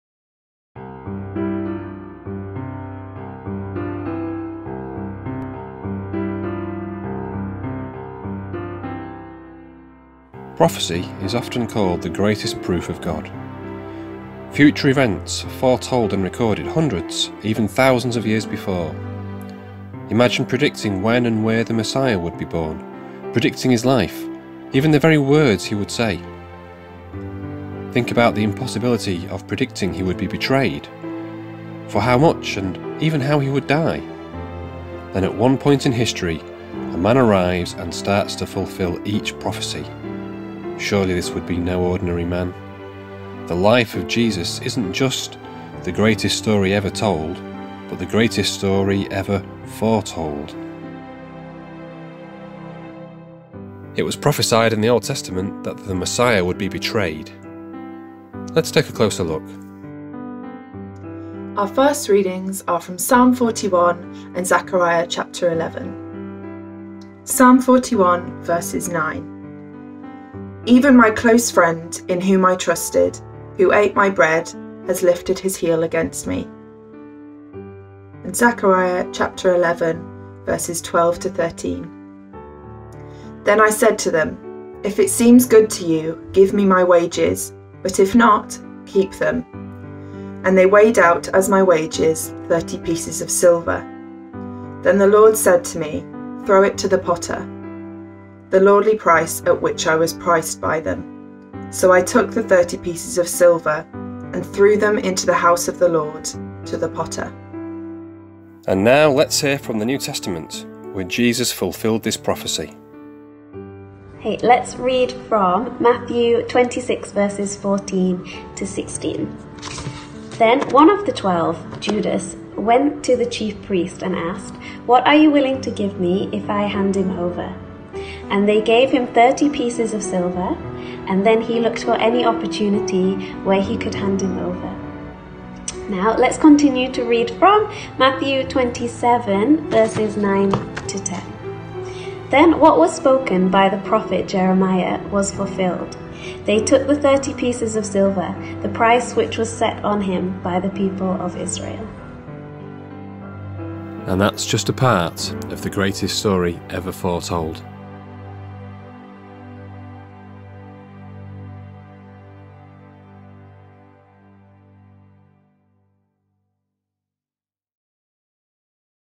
The Messiah Would Be Betrayed | THE GREATEST STORY EVER FORETOLD | GBC Online
The tenth in a series in which we see where the  prophecies about Jesus in the Old Testament were fulfilled in the New Testament.

Narrator: Neil Dunn
Readers:
Old Testament Prophecy: Kate Gray
New Testament Fulfilled: Liv Cahill